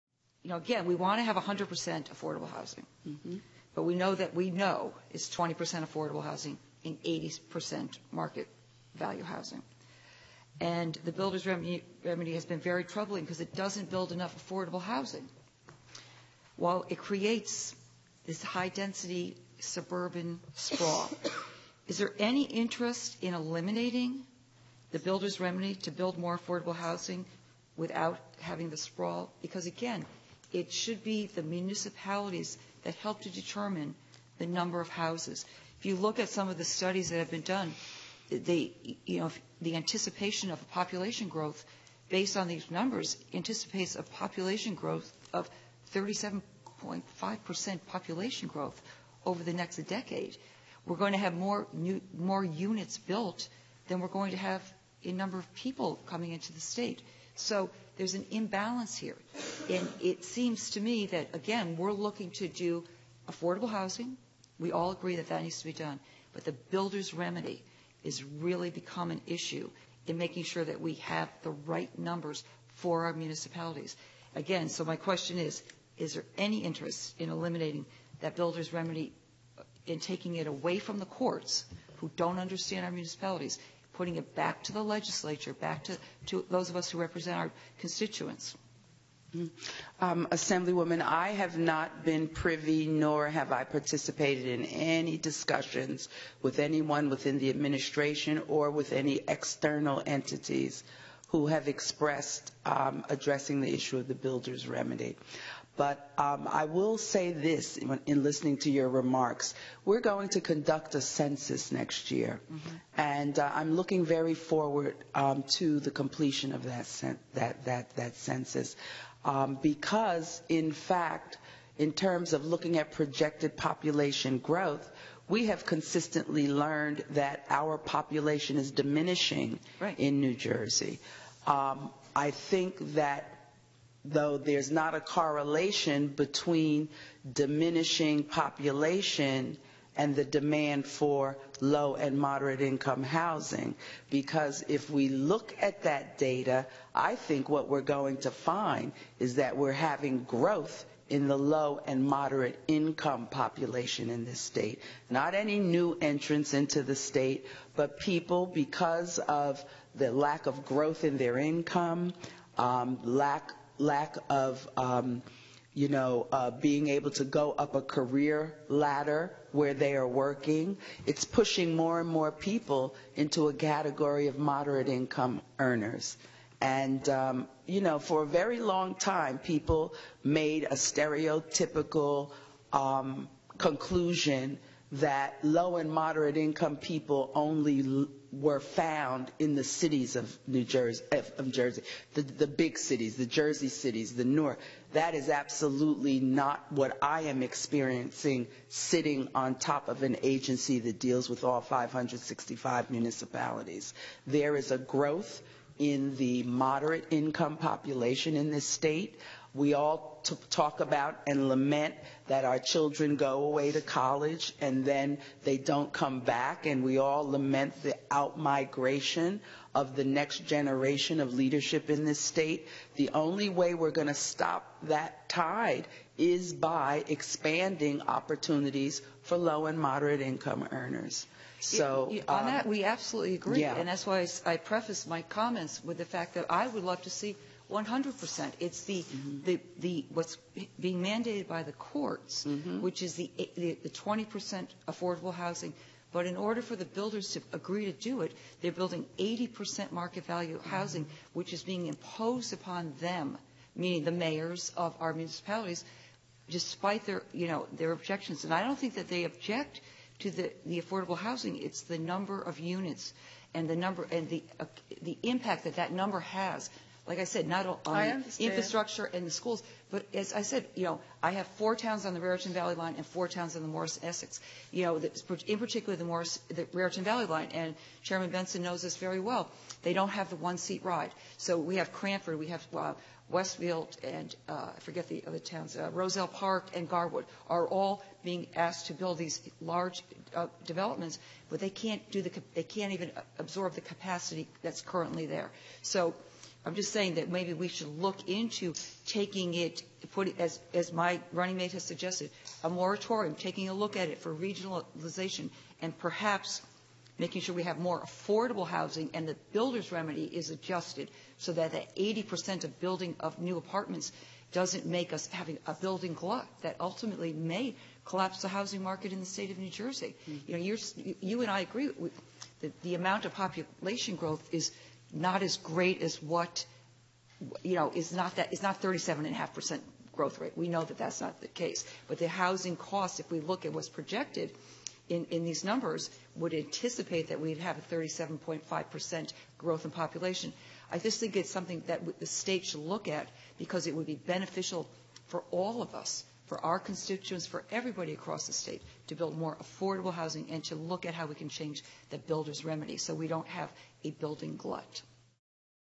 Munoz targets "builder's remedy" at budget hearing
Assemblywoman Nancy Munoz raised the problem of the “builder’s remedy” as a means of forcing municipal compliance with affordable housing obligations at an Assembly Budget Committee hearing in Trenton Monday.